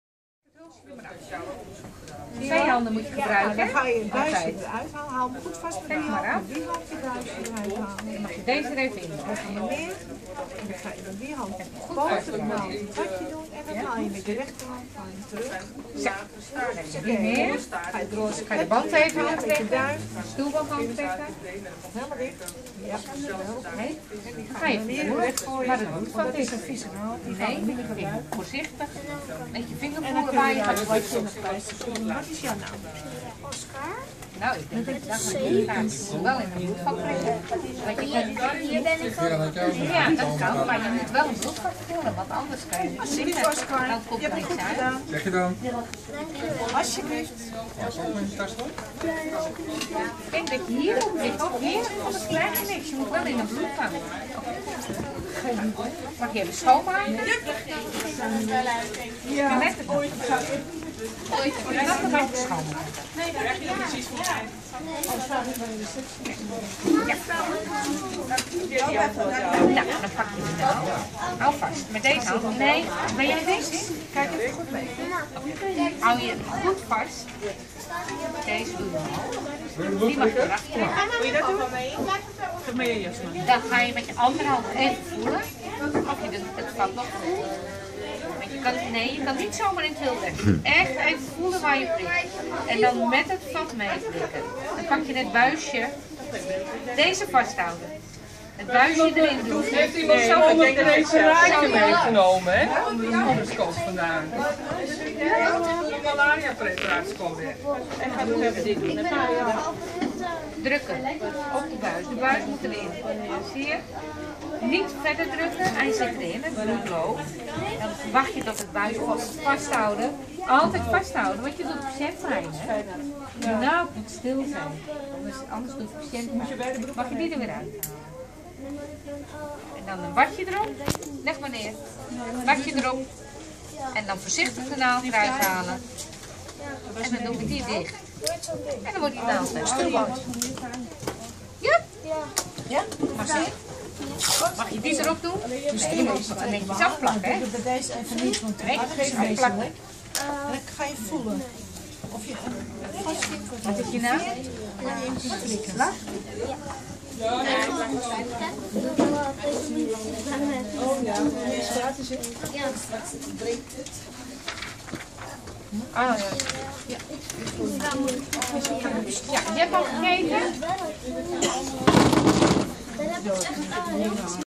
Open dag in het bovenij ziekenhuis.
Lars leert bloed prikken.